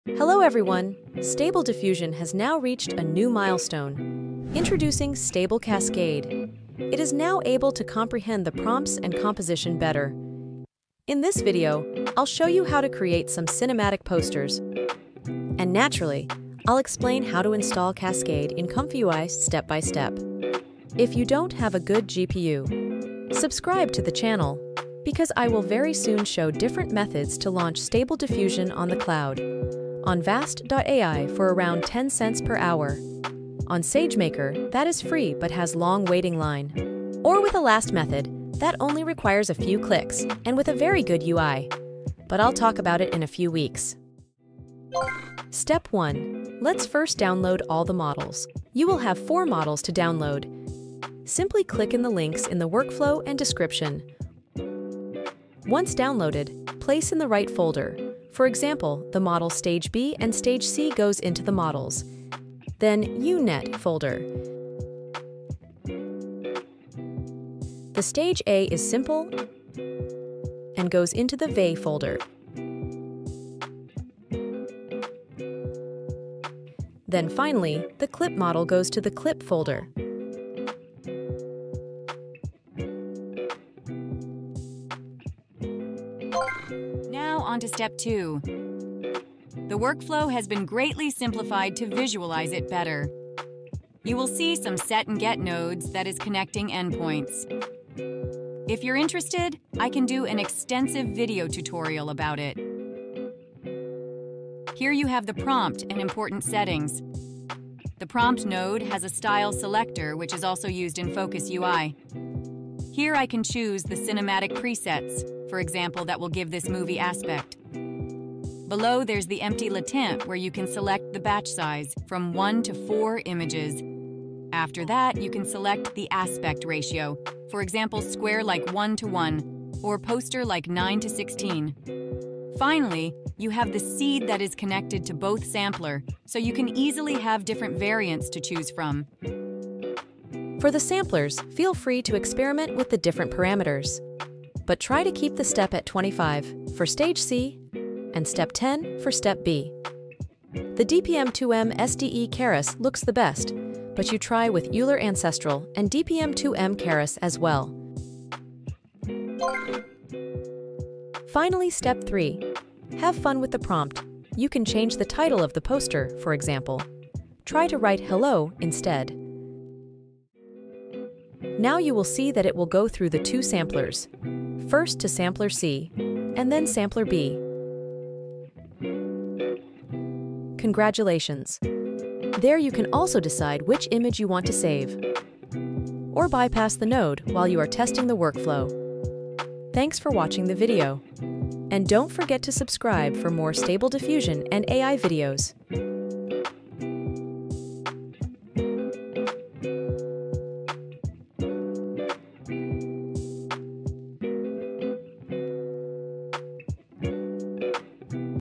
Better than Dalle and Midjourney ? Stable Cascade Install | ComfyUI Tutorials
Timestamps:
00:00 Introduction
00:30 Cloud GPU
00:44 Step 1 : Models
01:26 Step 2 : Worfklow
02:39 Step 3 : Prompts

I was just playing around with Stable Cascade earlier and made some movie poster to test the composition and letter writing. For info, there were no post-production, detailer or upscaling (just the cascade stages).
I've also simplified the workflow to better visualize the stage C and B.

Base Prompt :
Epic movie poster with
portrait of panther with glow light,
in the bottom a big text title "WILD JUNGLE" written on it,
the poster is neon cyberpunk cinematic style,